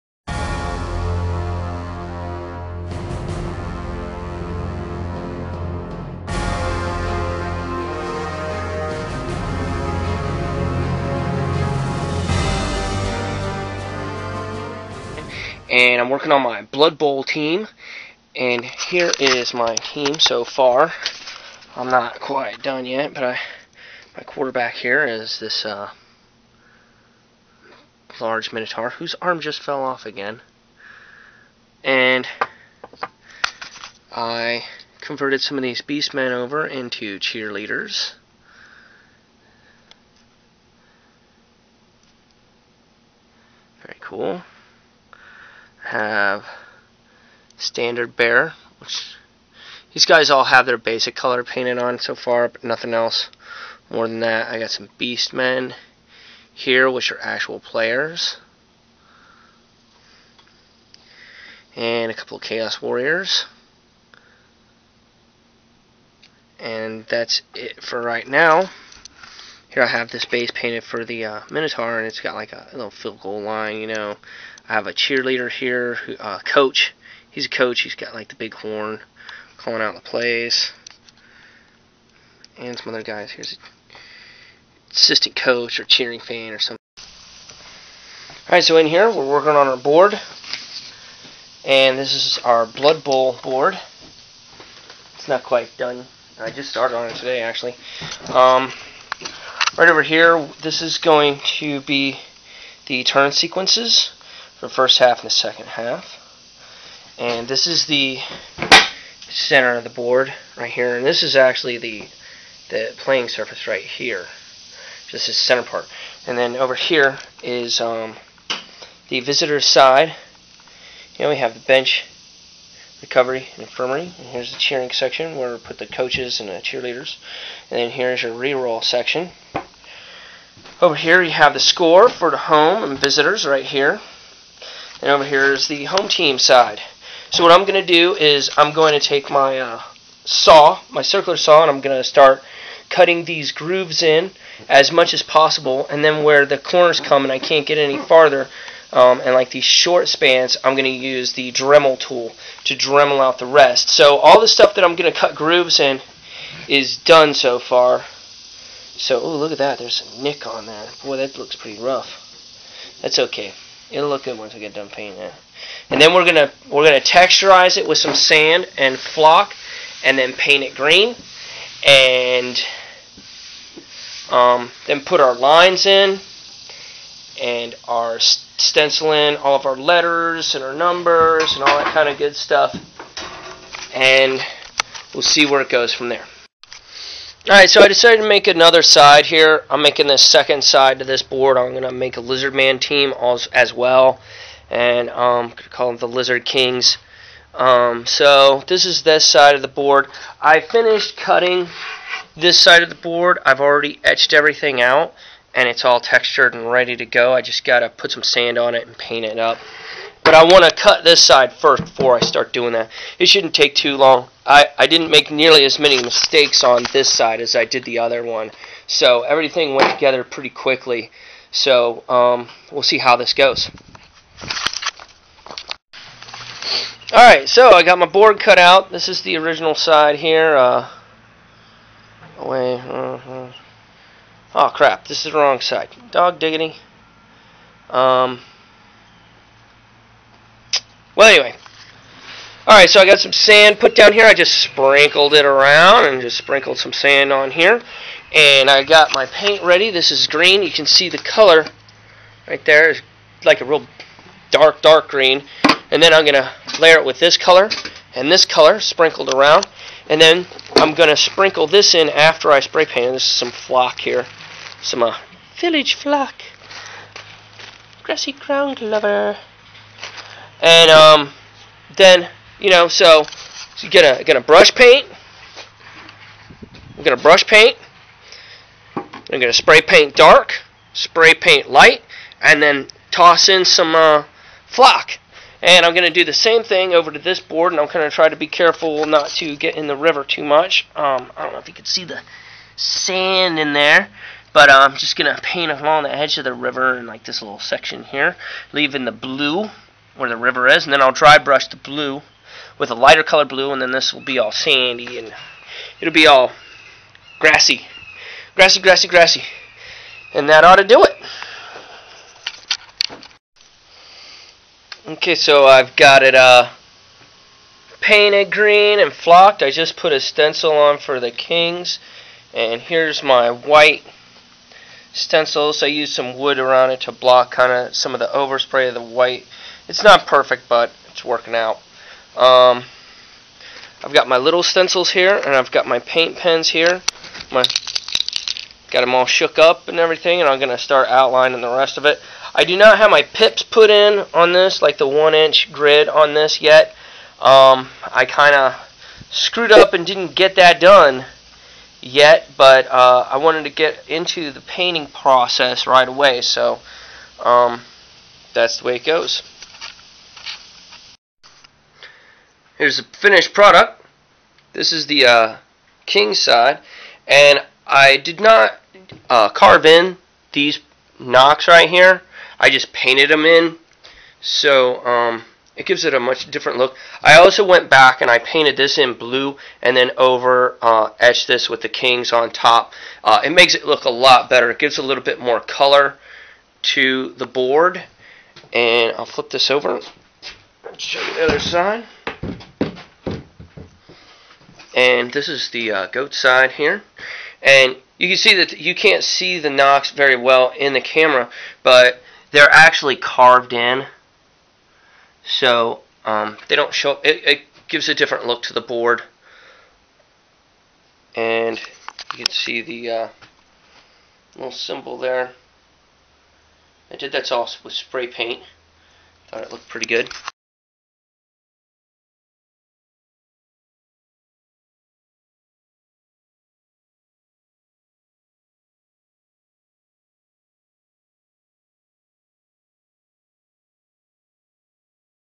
Blood Bowl
Build your own Blood bowl pitch from scratch.